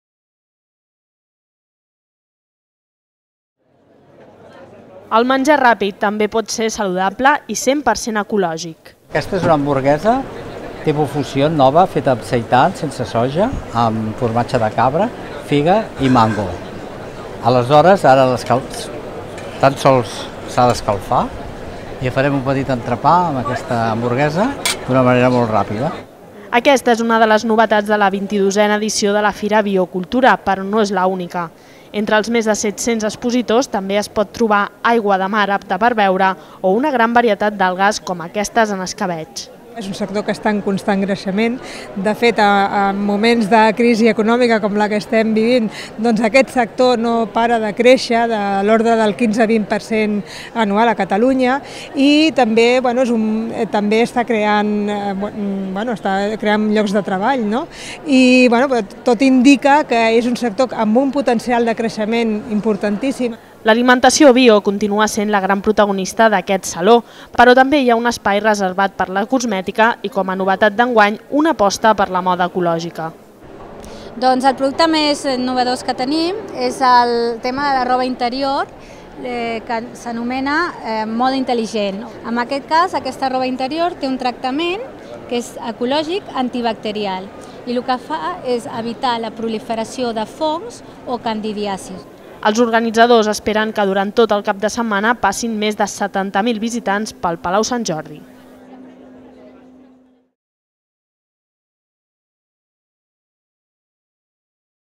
BioCultura omple de productes ecològics el Palau Sant Jordi
La fira de productes ecològics i consum responsable BioCultura torna a Barcelona. El saló, que enguany celebra la XXII edició, compta amb la participació més de 700 expositors i espera rebre més de 70.000 visitants. Una xifra que posa en evidència la bona salut del sector. Més informació